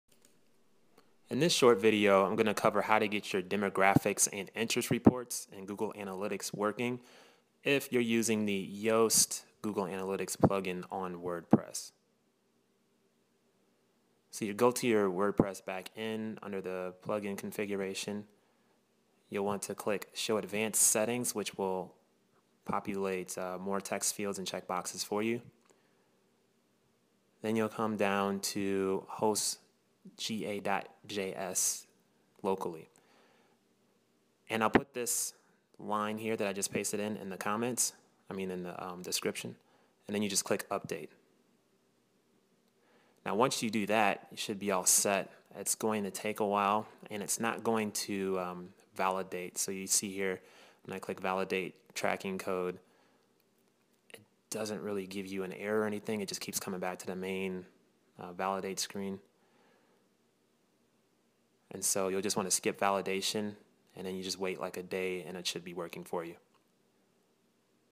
How to enable Demographic and Interest Reports using the Yoast Google Analytics for Wordpress Plugin
Copy and Paste the URL below to place where mentioned in the video.

It took about 20 minutes to figure out how to do this, while scouring different sites as Yoast doesn't have official documentation for this yet. I was able to find this fix on the Wordpress Forum at:

So I figured, I'd throw together this quick vid to save you some time. Thumbs up if it helps.